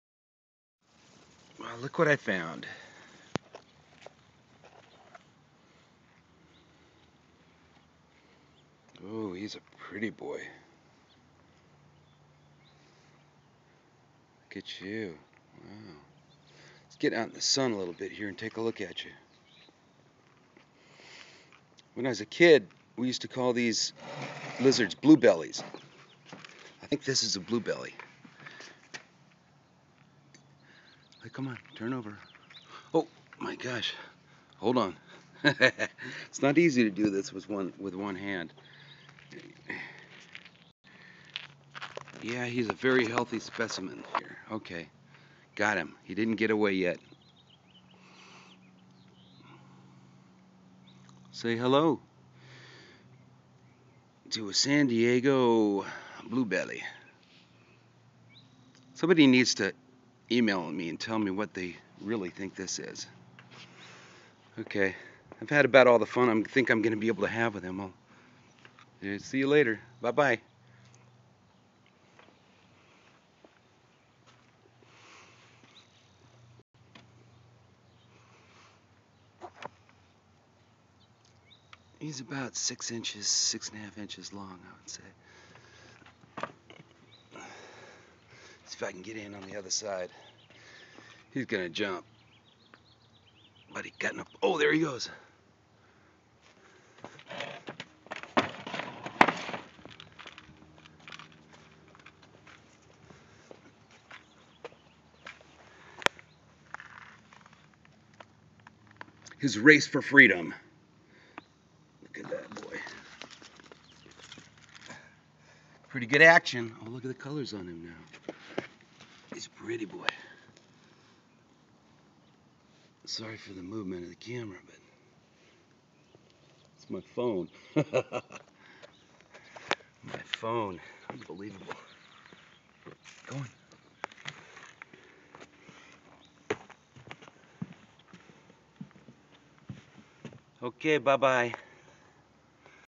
My San Diego LIzard Catch
This video was uploaded from an Android phone. I caught this beautiful lizard and did my best to handle him with one hand and record with the other